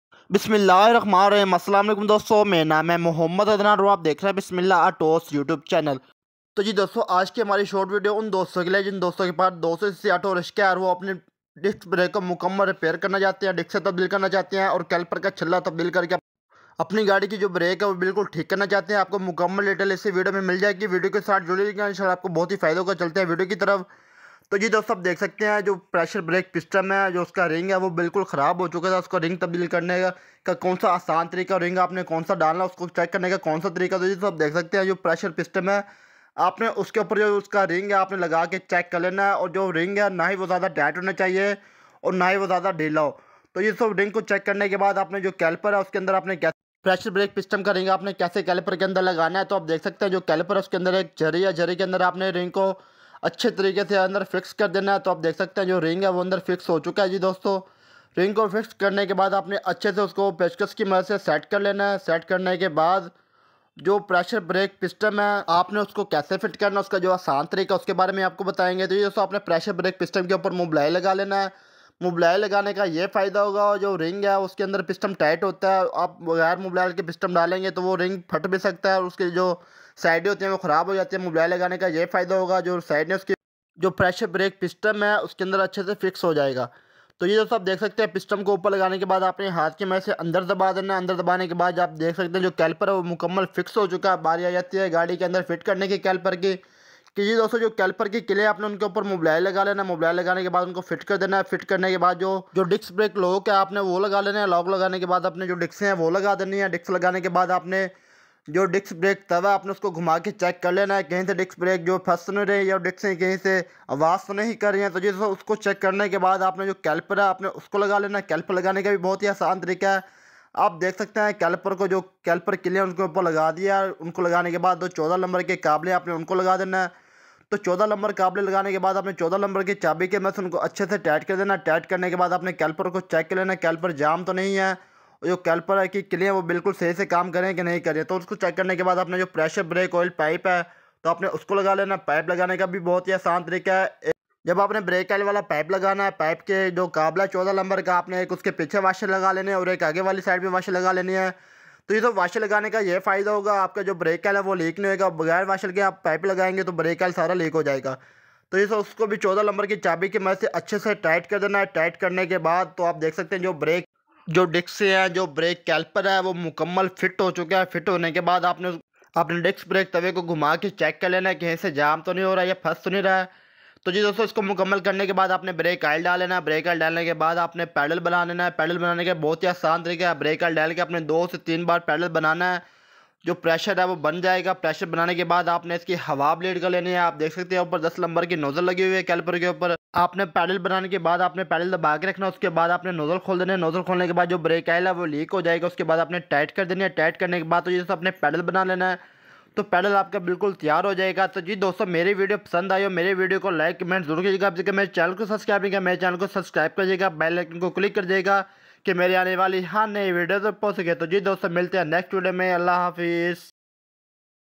AUTO RAKSHAK disc brake problem solve ||how to repair disc brake complete detail
how to replace disc brake and pressure break auto rickshaw 200 cc CNG
disc break aur repair karne ke mukmmal detail aapko isase video mein mil jayegi